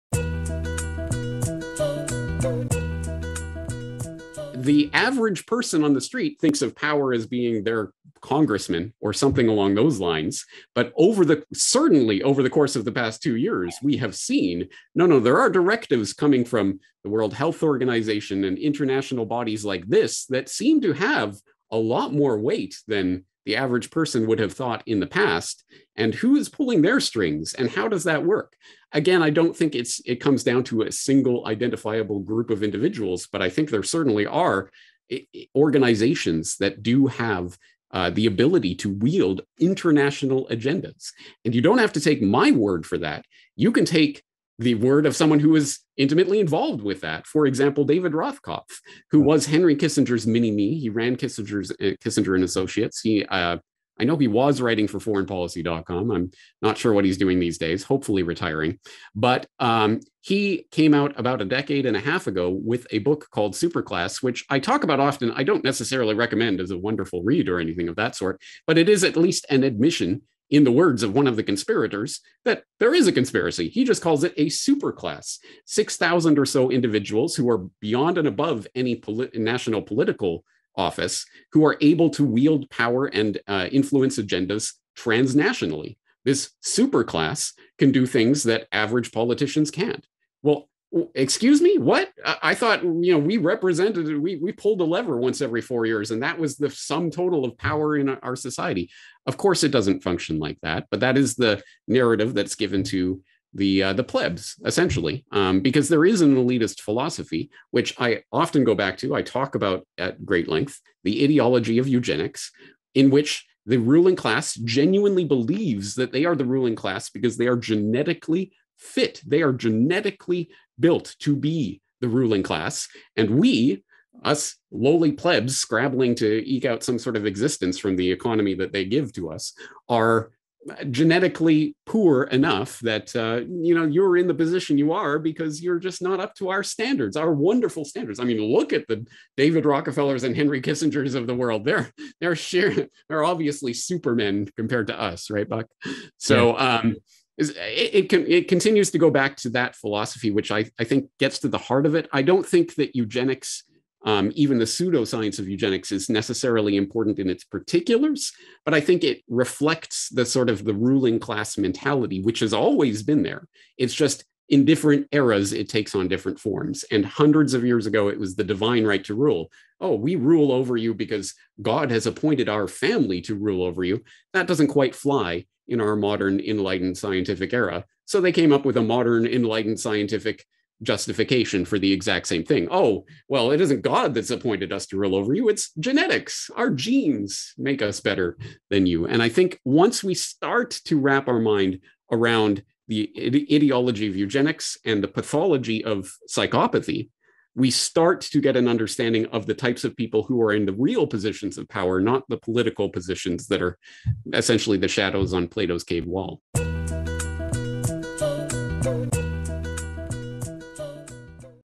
Those Who Have the Power
James Corbett talks about those who are actually pulling the strings, and the history of those people considering themselves to have either divine right to rule or just being genetically superior to all of us lower class folk.